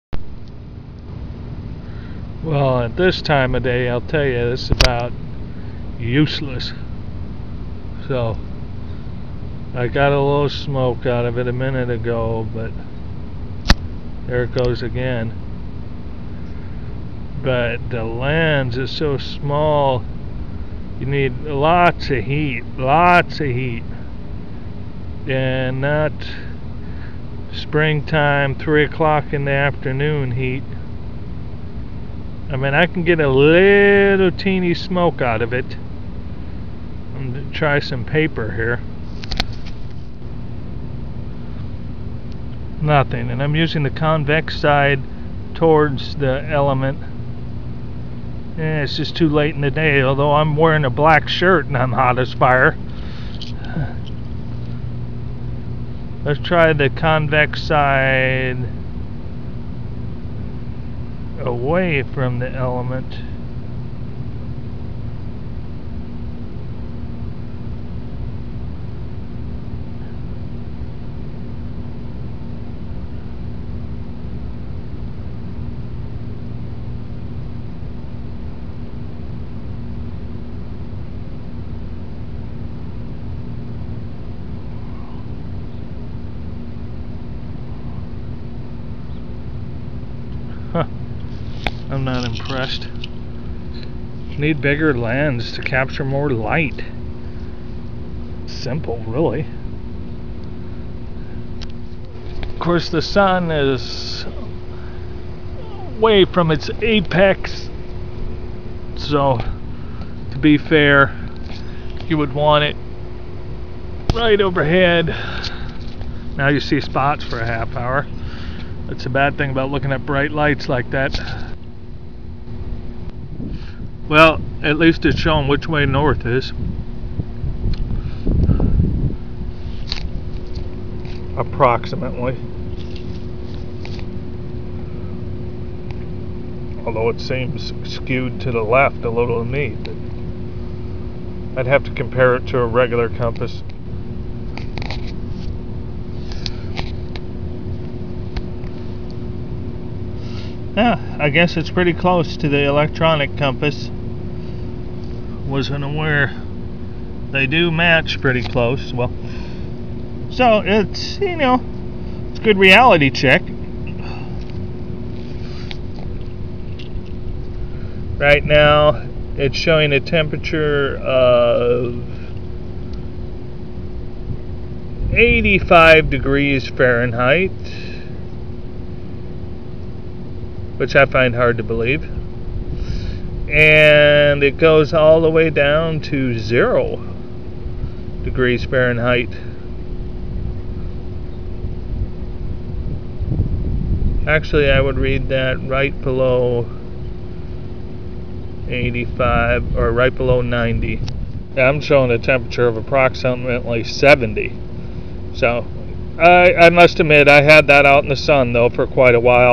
Coghlans Whistle Magnifier Compass Thermometer Walmart
Quick Review item Walmart Bushcraft run #3 cheap kit